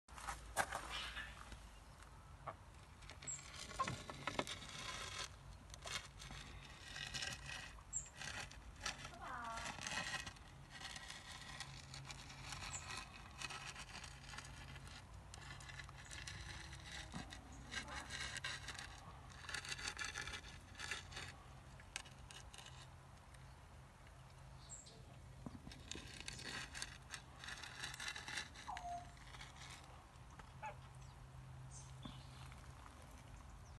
Stag Horn Thumb Stick Saluki, Falcon Desert Scene
I loved painting this scene it was so different from my usual subjects. It is rural and has animals too so what is there not to like.